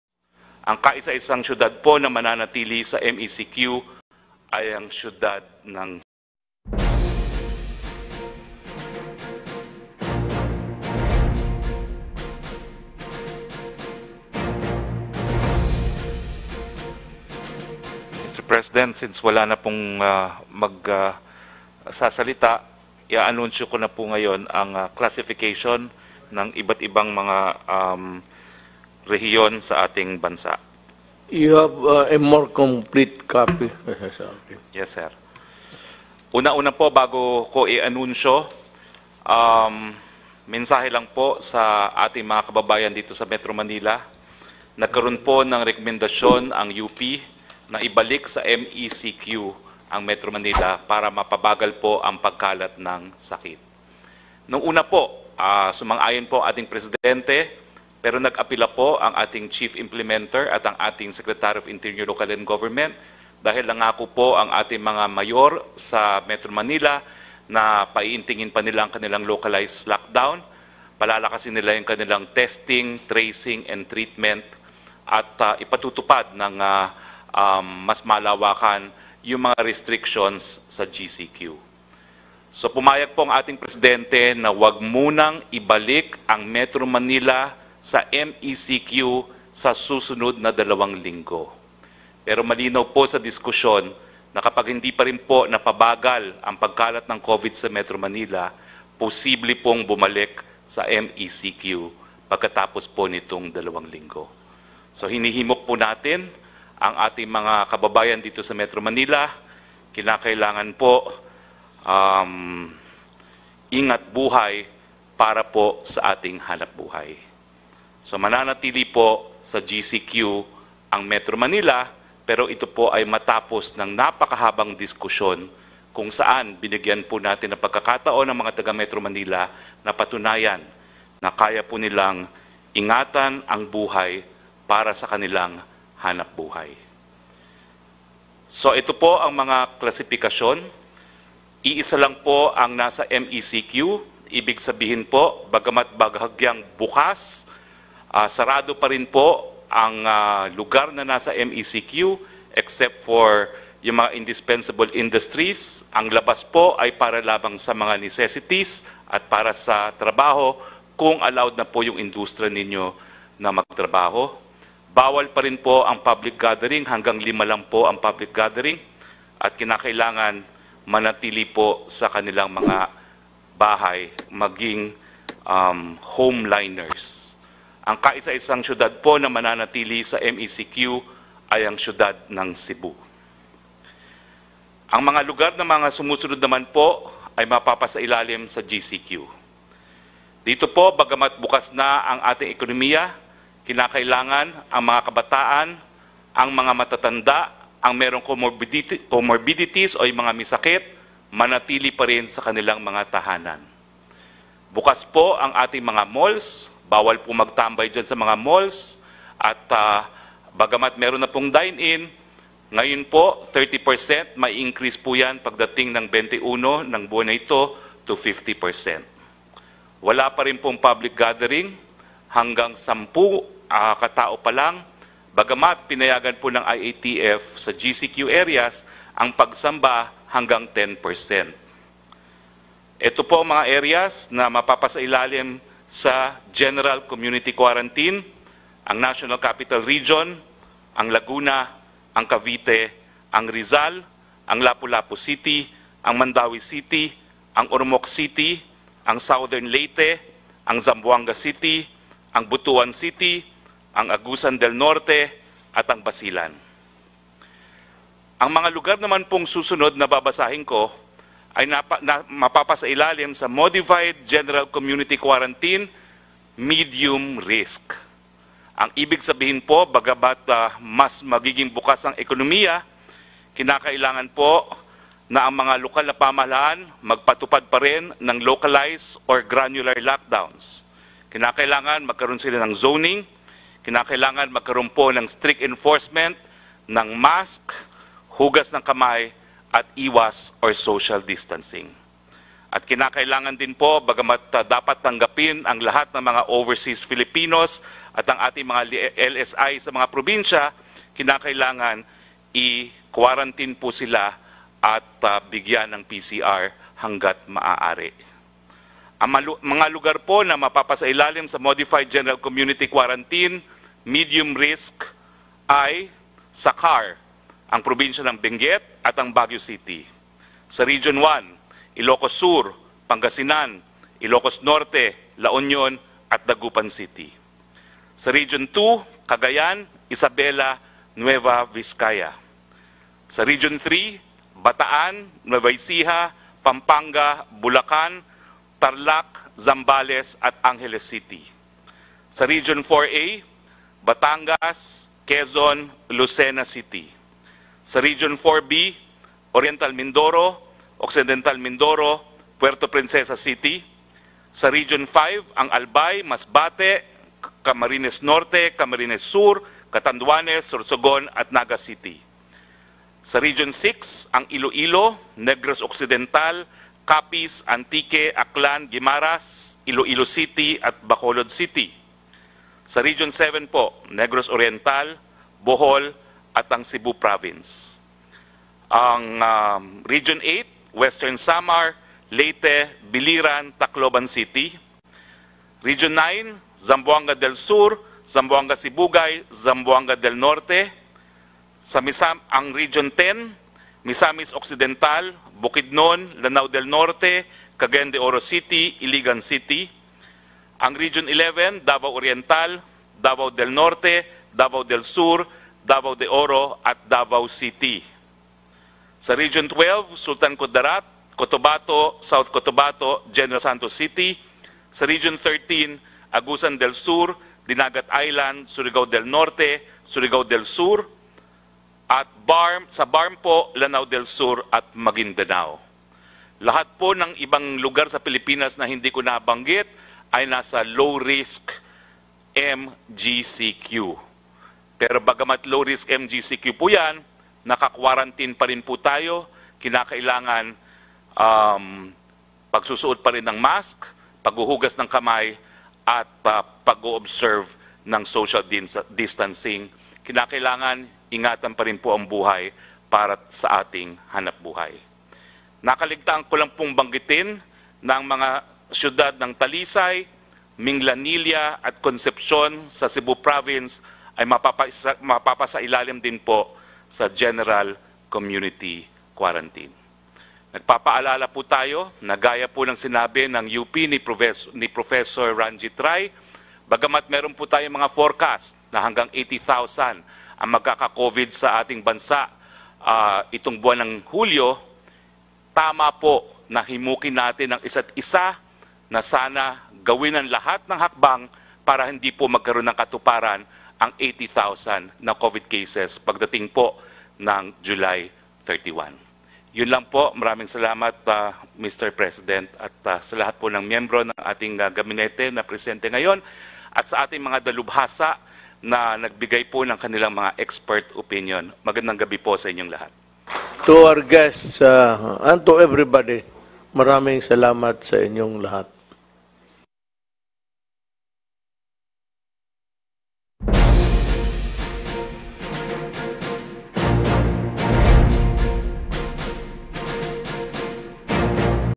JULY 15, 2020: METRO MANILA BALIK MECQ? | QUARANTINE CLASSIFICATIONS OF PROVINCES ALREADY ANNOUNCED
This is LUYVITTON Channel